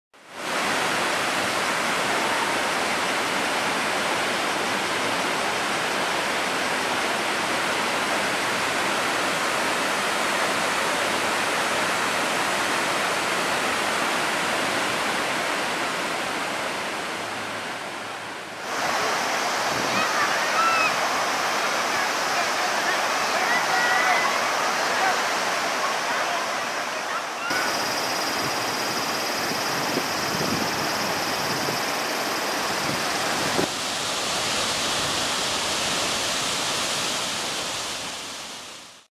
玖珠・九重 滝めぐり
大分県玖珠町・九重町には多くの滝があります。今回の帰省では慈恩の滝、龍門の滝、三ケ月の滝、西椎屋の滝 4か所の滝をめぐってきました。この他にも多くの有名な滝がありますので皆さんも是非どうぞ、現在ホテル・旅館など最高7割引きの復興支援があります 活用ください。